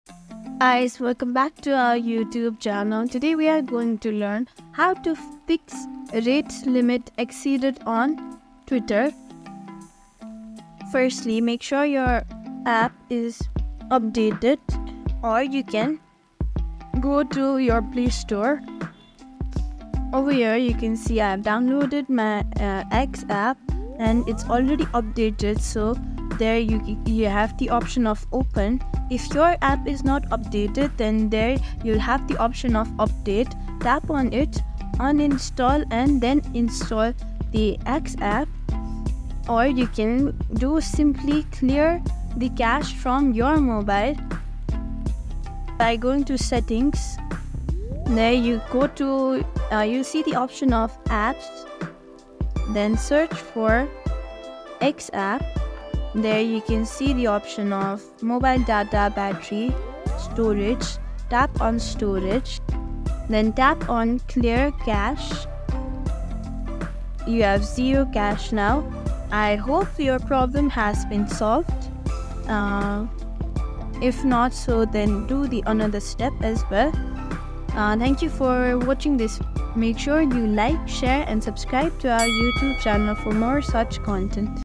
How To Fix Rate Limit Exceeded on (Twitter) X 2024 ?
Frustrated by the 'Rate Limit Exceeded' error on X? Our 2024 tutorial provides you with practical solutions to fix this issue and get back to tweeting without interruptions. Follow these steps to understand what causes the rate limit and how to avoid it in the future. Watch now and keep your X experience smooth and hassle-free!